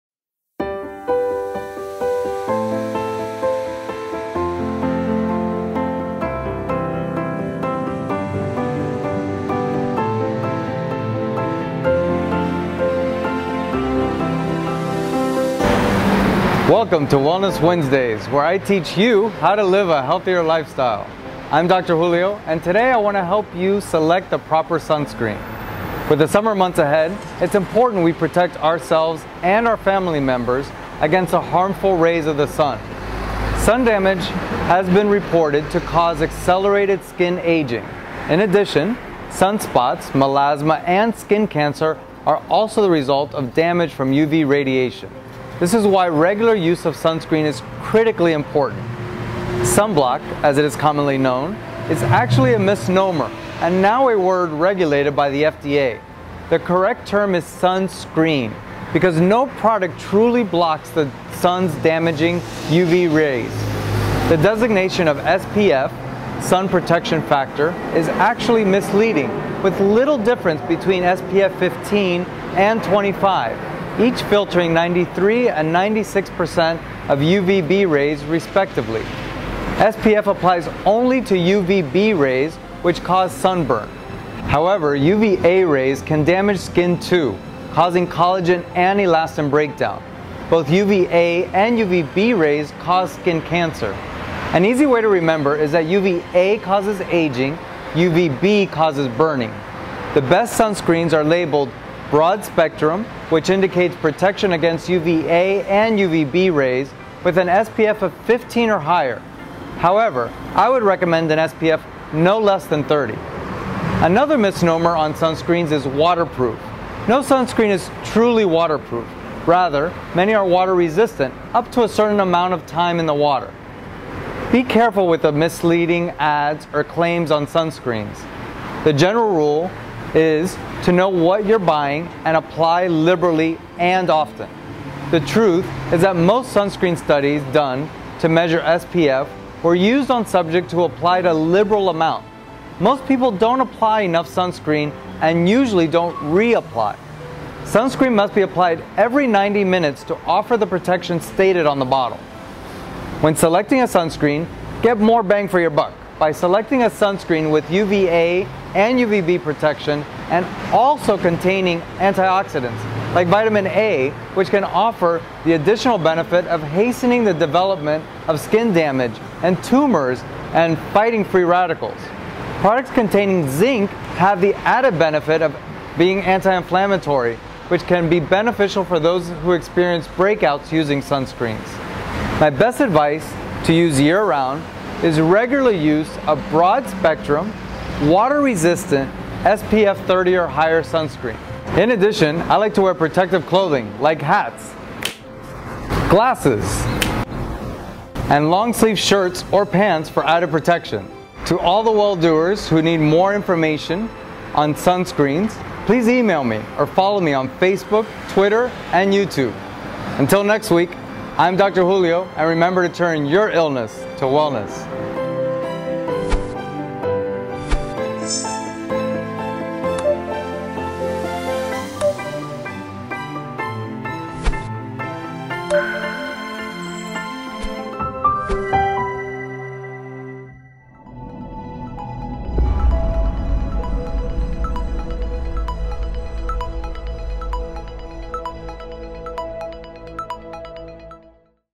Facts about SUNSCREEN & SPF - Dr Julio Hernandez
Biscayne Wellness Center

SPF stands for Sun Protection Factor. The number is determined experimentally indoors by exposing human subjects to a light spectrum meant to mimic noontime sun. Some subjects wear sunscreen and others do not. The amount of light that induces redness in sunscreen-protected skin, divided by the amount of light that induces redness in unprotected skin is the SPF. It is mainly a measure of UVB protection and ranges from 1 to 45 or above.